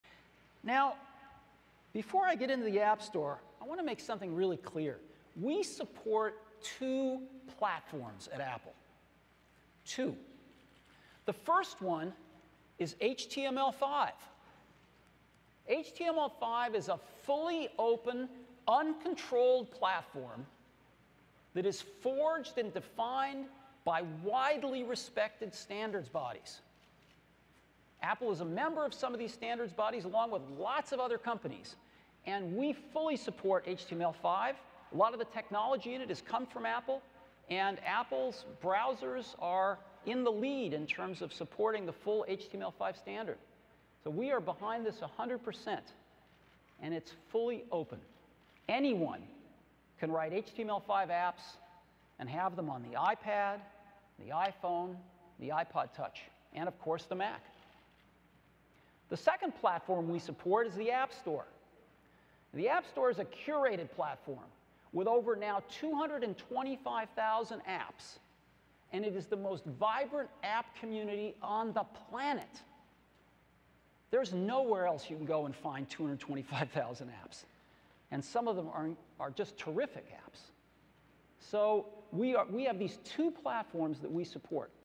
WWDC 2010: Steve Jobs on the platforms
We support two platforms at Apple. Two. The first is HTML5, a fully-open, uncontrolled platform that is forged and defined by standards bodies. We fully support HTML5. Apple's browsers are in the lead in terms of supporting the full HTML5 standard, and we are behind this 100%. It is fully open."